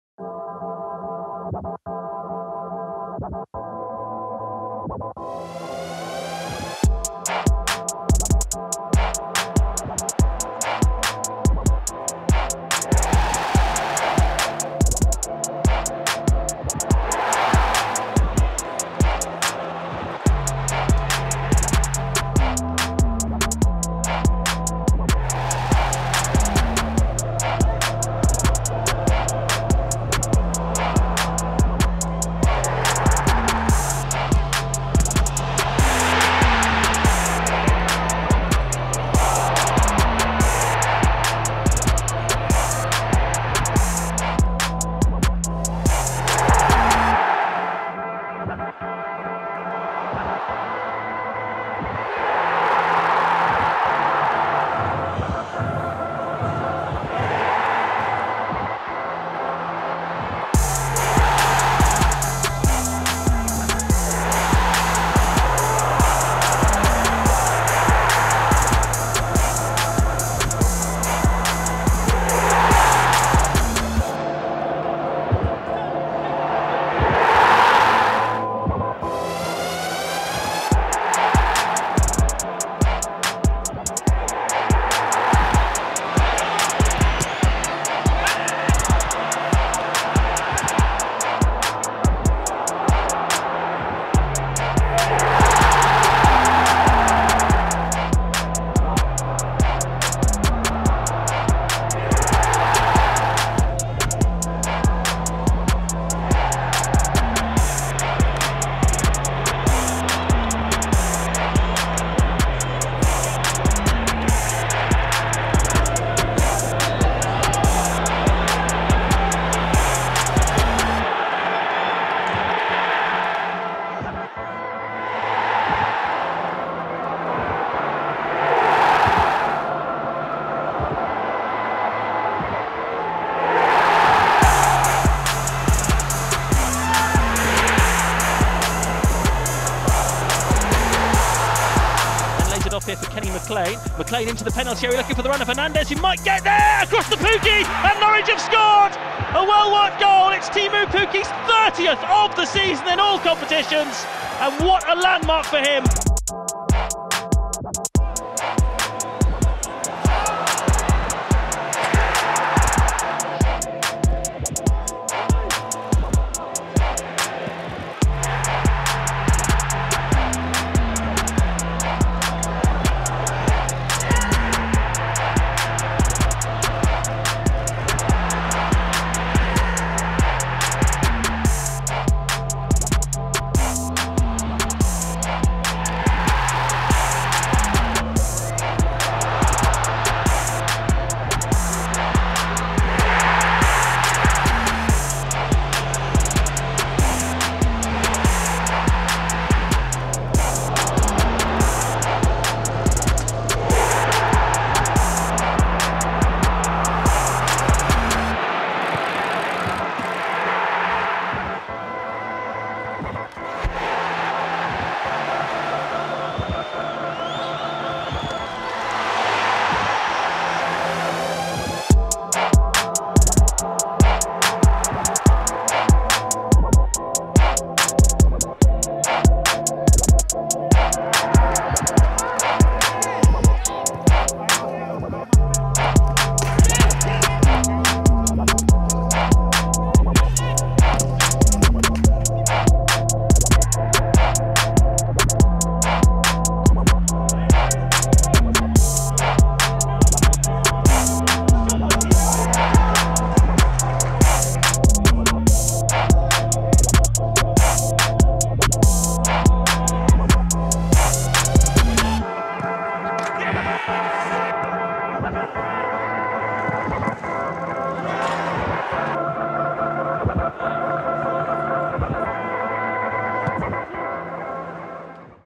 TEEMU PUKKI | ALL 50 GOALS FOR CITY 🐐
Sit back, relax and enjoy all of Teemu Pukkis FIFTY (yes that is right) FIFTY goals for Norwich City, in 100 appearances.